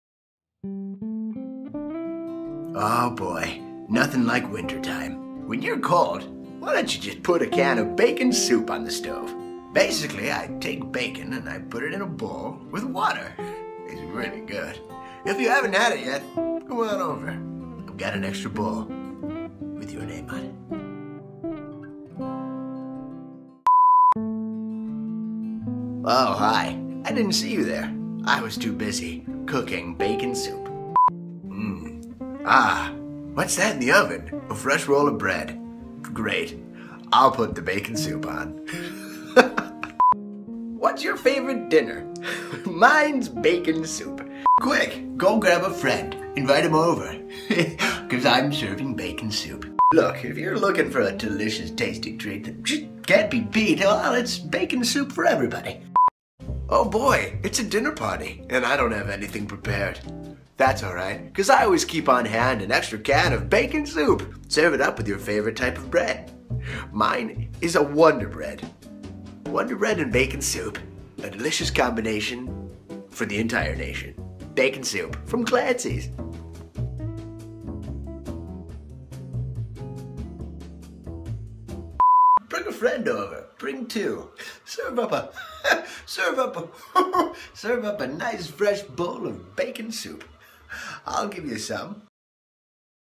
Bacon Soup Commercial
What's that in the oven? It's a fresh roll of bread. Great. I'll put the Bacon Soup on.

This winter season is colder than ever. Stay warm, my friends, with a hot bowl of Bacon Soup. From Clancy's kitchen, to your home. It's Bacon Soup.